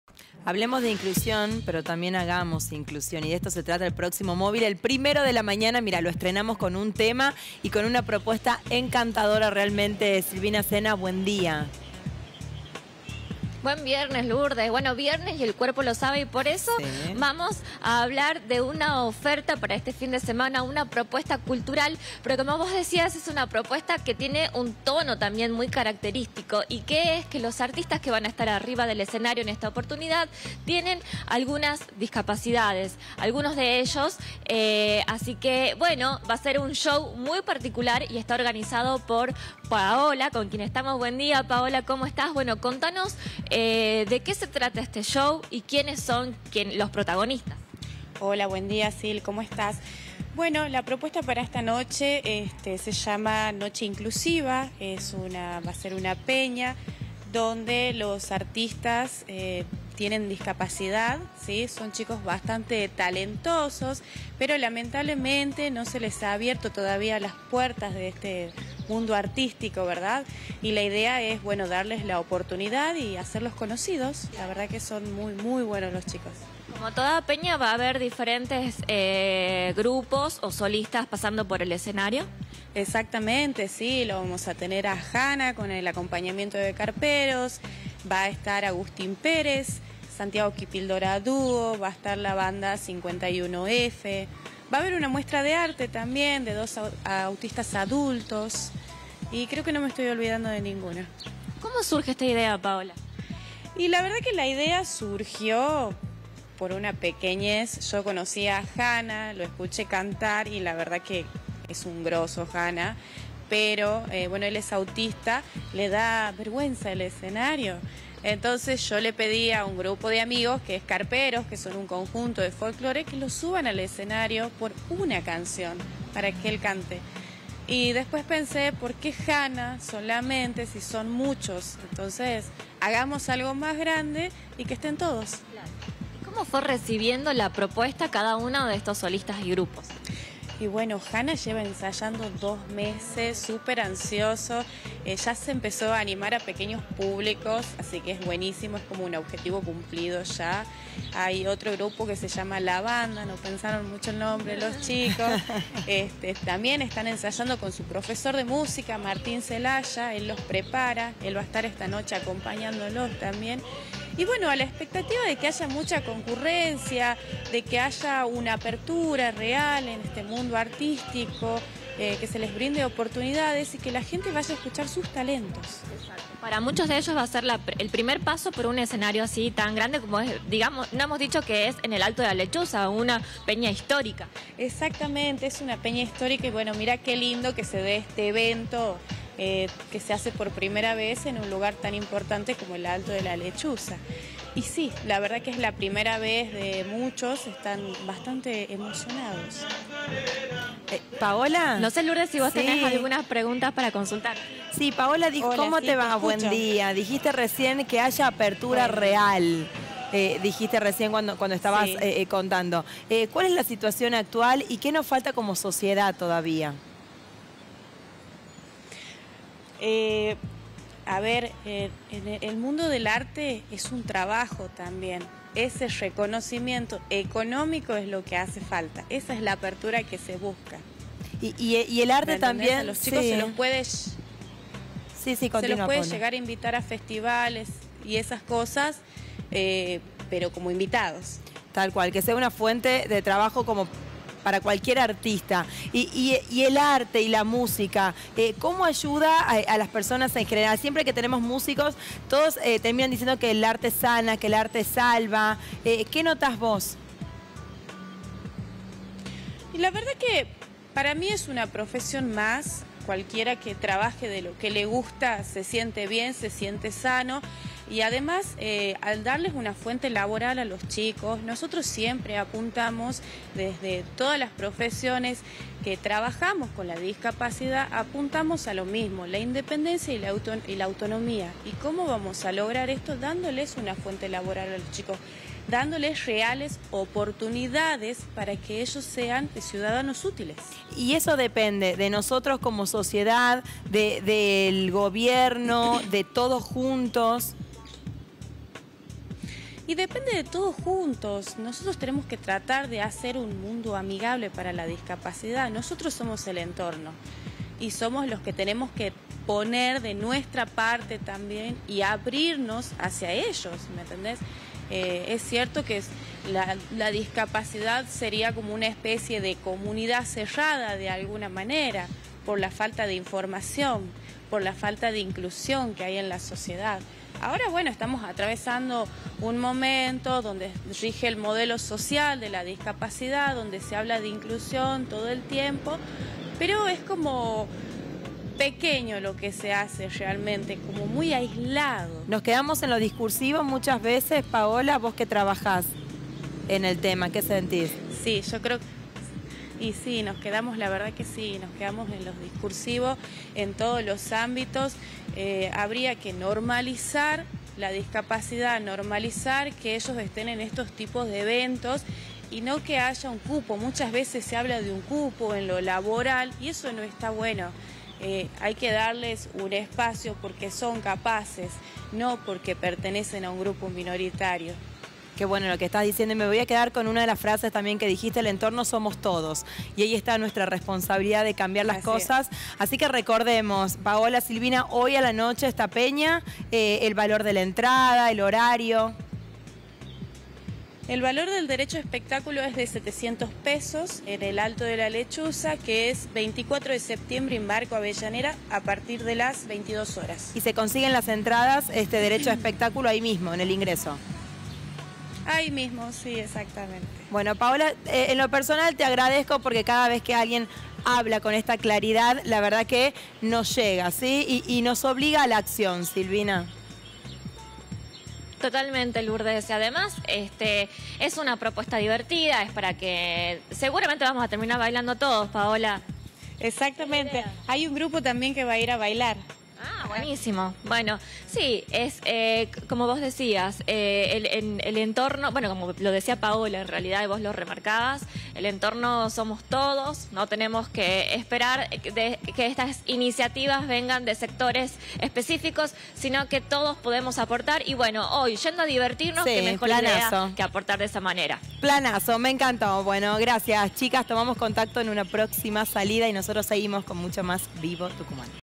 Peña y folclore: hoy se realizará la “Noche inclusiva” en el Alto de la Lechuza
Para hoy se espera una noche inclusiva en el Alto de la Lechuza a puro folclore. La peña comprende a artistas con discapacidad sumamente talentosos que tendrán la oportunidad de darse a conocer.